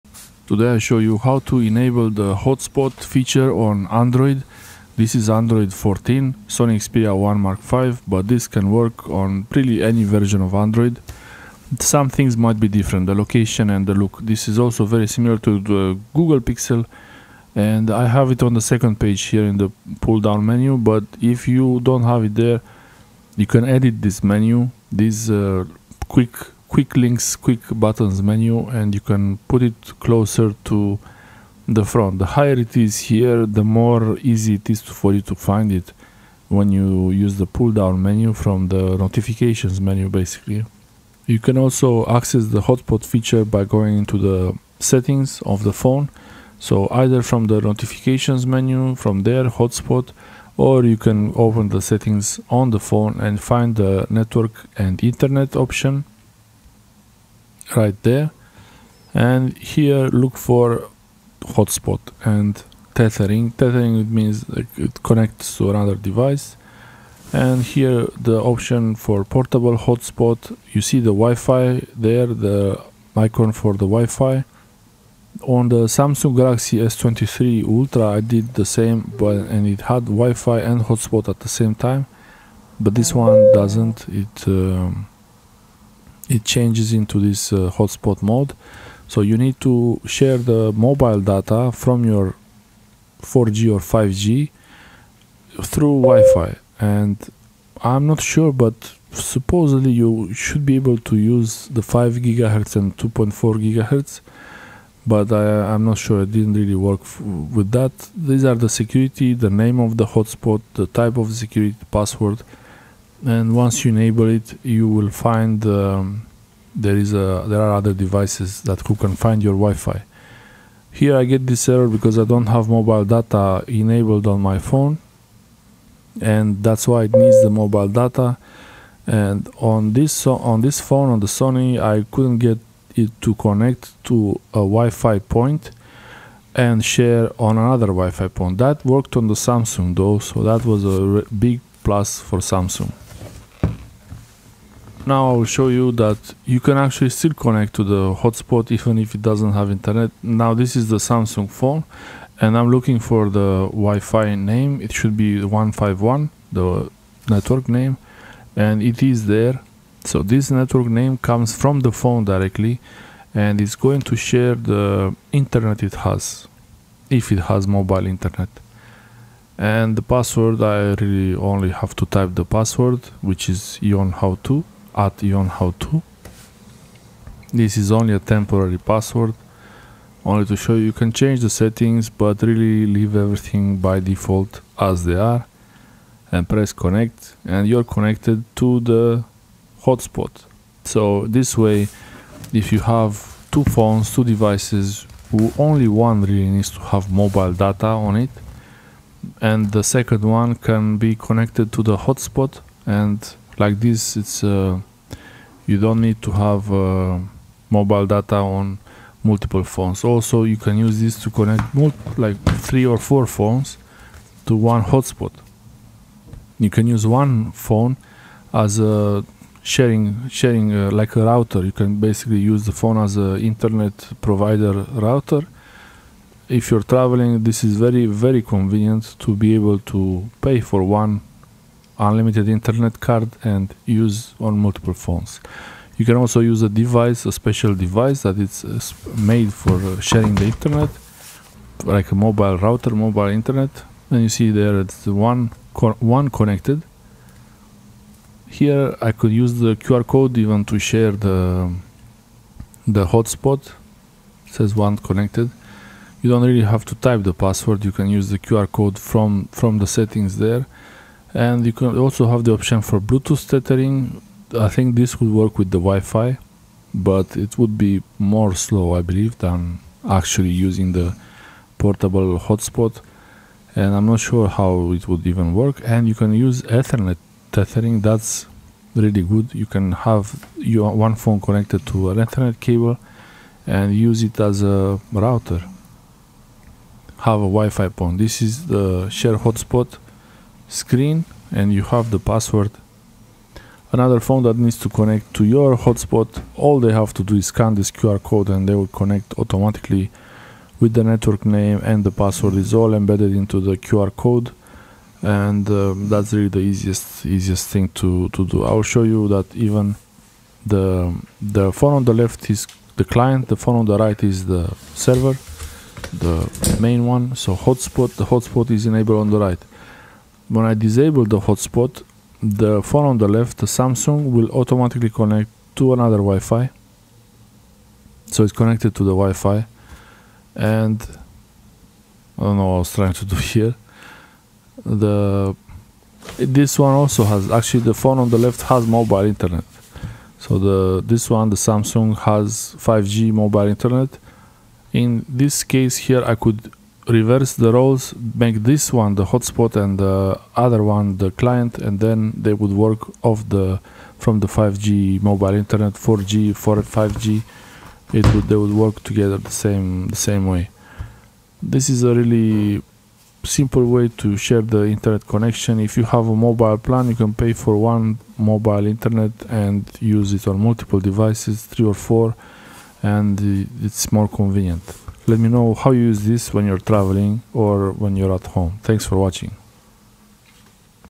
Android Enable Mobile Hotspot to Share Internet
How to Enable and Use Mobile Hotspot on Android Devices.

How to enable mobile hotspot on Android and use a Android phone as a mobile hotspot. Set up Wi-Fi hotspot on Android tablets.

In this video, I'm going to show you how to enable and use the mobile hotspot feature on your Android phone or tablet. A mobile hotspot lets you share your device's internet connection with other devices, like laptops and other phones, making it incredibly useful when you need internet access on the go.

First, open the "Settings" app on your Android device. Scroll down and tap on "Network & Internet" or "Connections," depending on your device. Next, tap on "Hotspot & tethering" and then "Wi-Fi hotspot." Toggle the switch to turn on your mobile hotspot. You can customize your hotspot settings by tapping on "Set up Wi-Fi hotspot," where you can change the network name (SSID) and password to something unique and easy to remember.

Once your mobile hotspot is enabled, you'll see a notification in the status bar. Other devices can now connect to your hotspot using the Wi-Fi network name and password you set up. To manage connected devices, go back to the "Wi-Fi hotspot" settings. Here, you can see which devices are connected and disconnect them if needed.

Any questions?